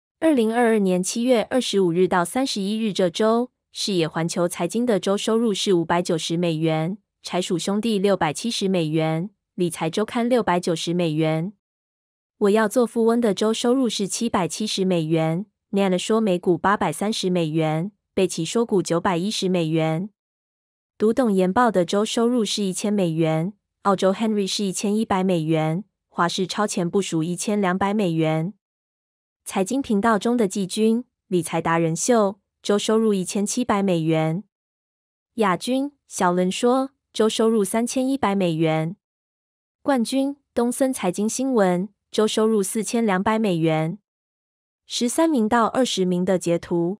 ‘財經頻道收入排行(2022.7.25-31) 東森財經新聞，小Lin说，理財達人秀，華視鈔錢部署，澳洲Henry，读懂研报，贝奇说股，NaNa说美股，我要做富翁，理財周刊，视野环球财经
按照每千次觀看2.5美元統一計價。粉絲活躍度是用日均觀看次數除以訂閲人數。
第13名到第30名分別是：13，郭思治工作室。14，90後創業家掃地僧。15，贝拉聊财金。16，狼王美股财经。17，理財不能等。18，SHIN LI。19，堆金積玉。20，美投讲美股。21，阳光财经。22，怪老子理財。23，94要賺錢。24，Better Leaf 好葉。25，蕾咪。26，金臨天下。27，CKBRO 置家兄弟。28，30歲財務自由。29，懶錢包。30，蒙面财经。
30個財經頻道7月25日-31日總流量比上週下降2.1%。